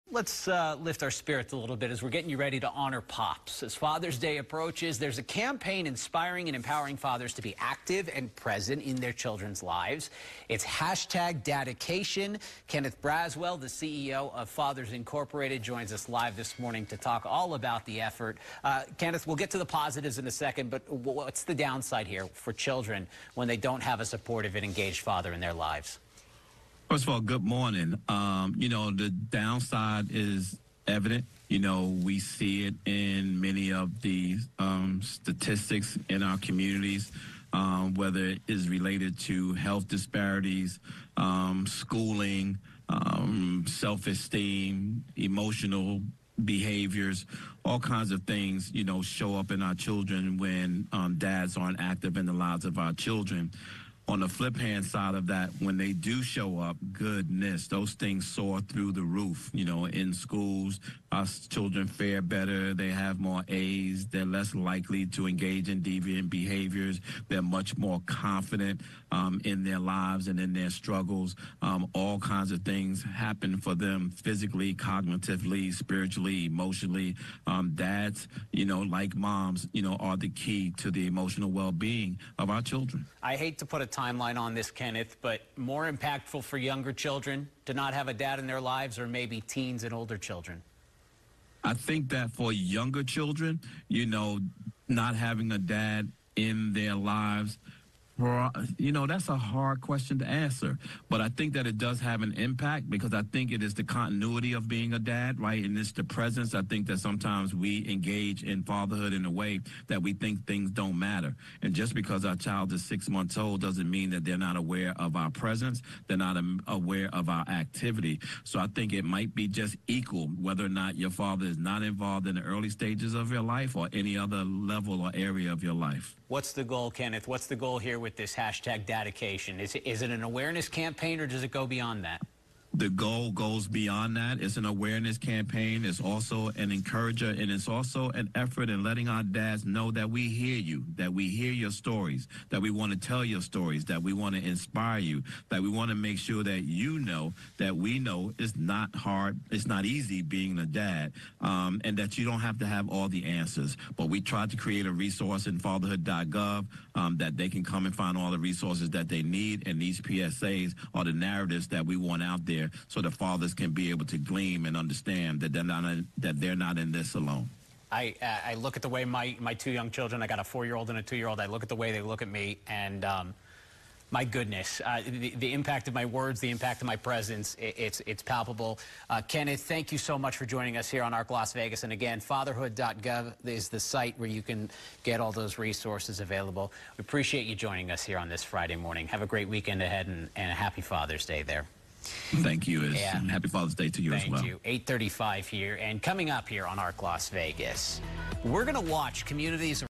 Kenneth Braswell on DADICATION w/ KVCW (IND Las Vegas)
In collaboration with the Administration for Children and Families (ACF) at the U.S. Department of Health and Human Services, and the National Responsible Fatherhood Clearinghouse (NRFC), the Ad Council has launched a new series of public service announcements (PSAs) aimed at celebrating and supporting responsible fatherhood.